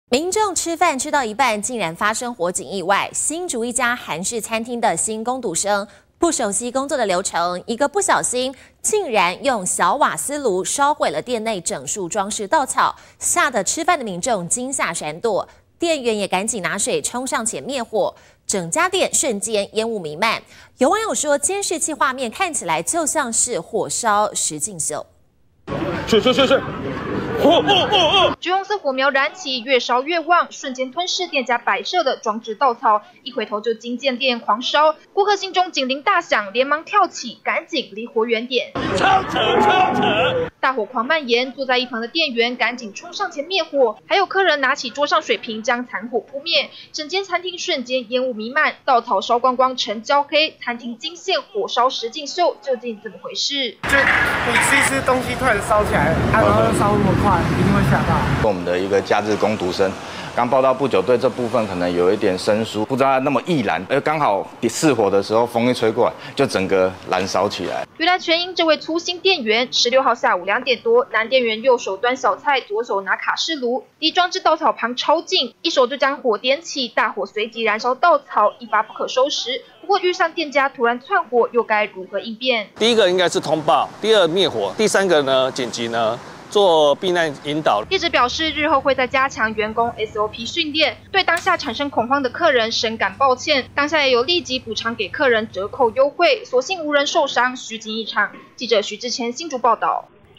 小瓦斯爐靠近裝置蘆葦草 驚魂！點火瞬間燃燒
民眾吃飯吃到一半，竟發生火警意外！新竹一家韓式餐廳的新工讀生，不熟悉工作流程，一個不小心，竟然用小瓦斯爐，燒毀店內整束蘆葦草。嚇得吃飯民眾，驚嚇閃躲，店員趕緊拿水，衝上前滅火，整家店瞬間瀰漫煙霧。有網友說，監視器影像猶如火燒實境秀。